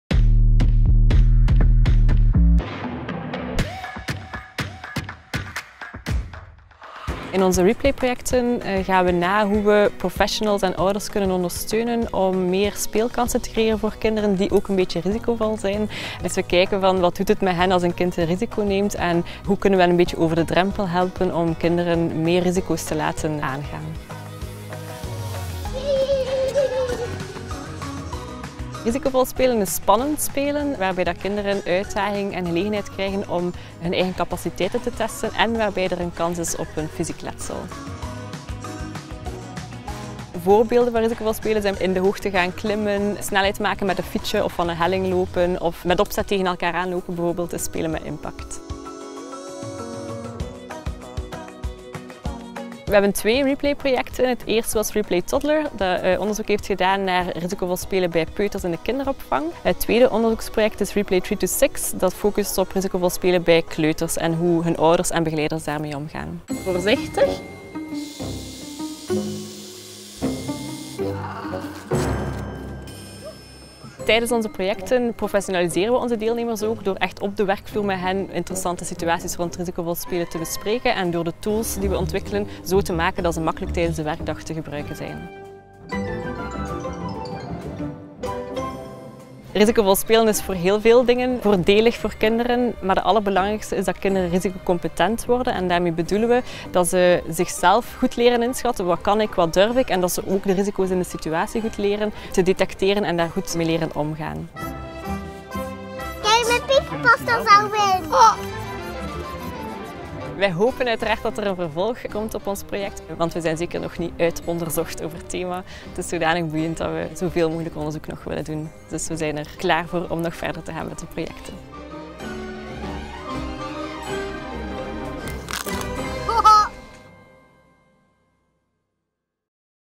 REplay Toddler, risicovol spel bij peuters
Het klinkt misschien vreemd... maar de speelomgeving té veilig maken, houdt ook risico's in voor kinderen. Benieuwd hoe dat precies zit? In dit 2 minuten-filmpje legt Helena het uit!